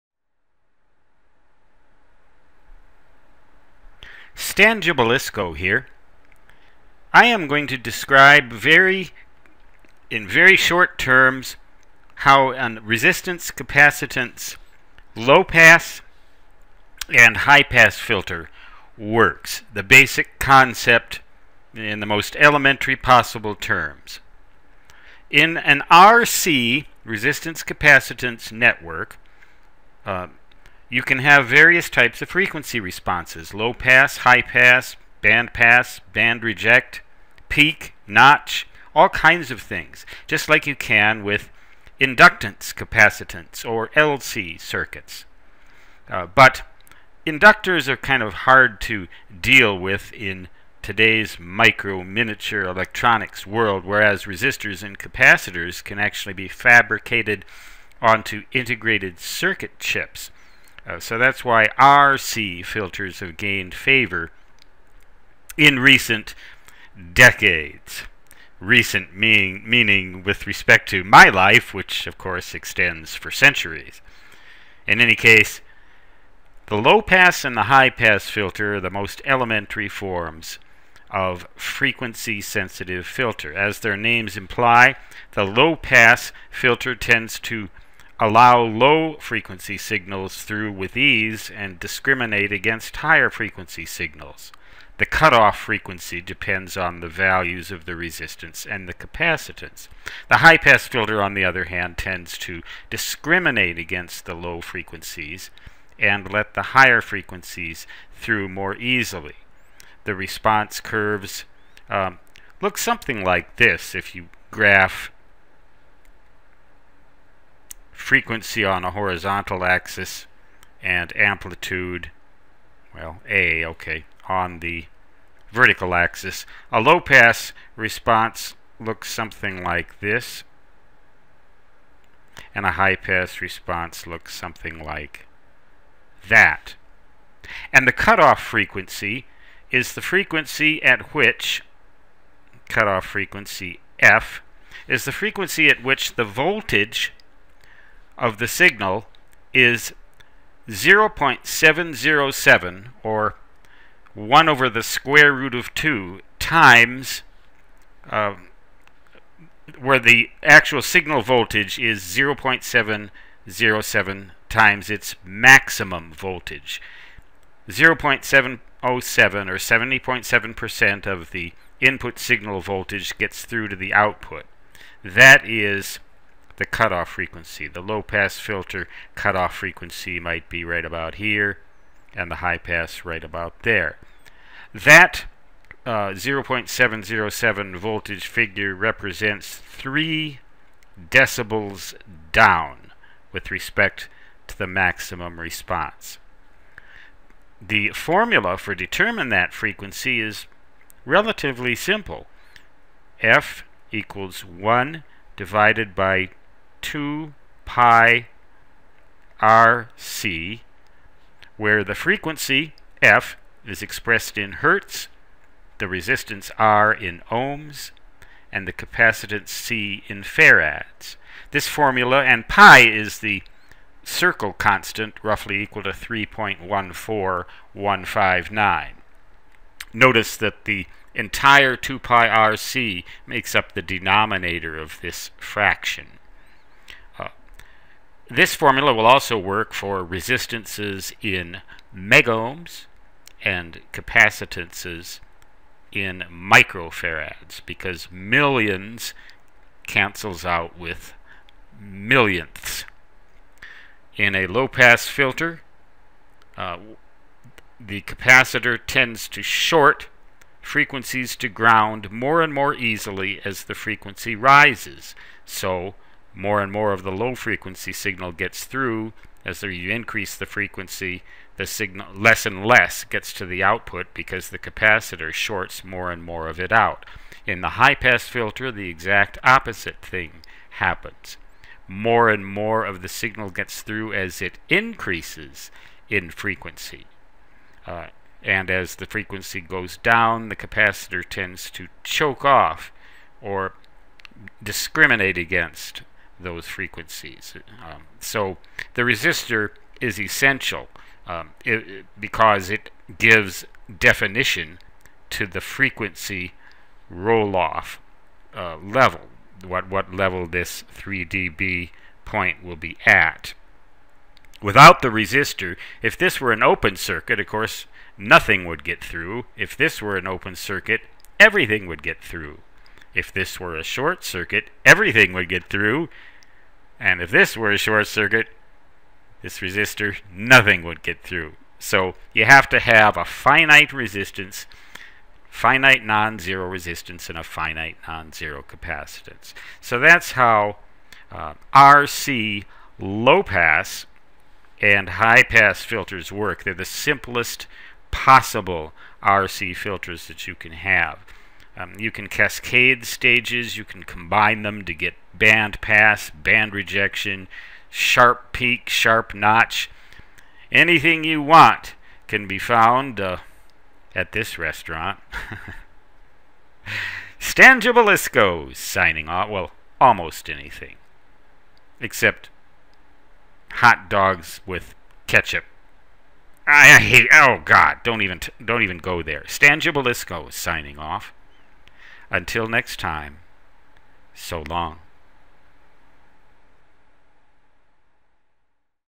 How Simple RC Filters Work
Here's a glimpse at the dark magic behind the functioning of resistance-capacitance (RC) lowpass and highpass filters.